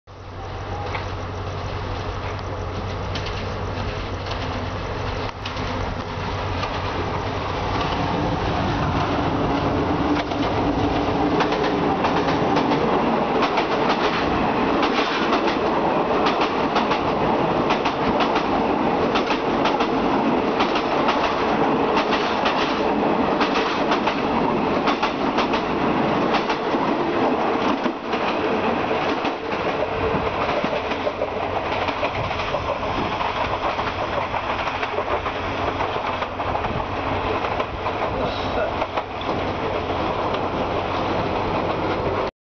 140 รอหลีก 141 ที่สถานีปางอโศก
รถเร็วที่ 140 อุบลราชธานี-กรุงเทพ รอหลีก รถเร็วที่ 141 กรุงเทพ-อุบลราชธานี ที่สถานีปางอโศก จ.นครราชสีมา[สถานีรถไฟปางอโศก]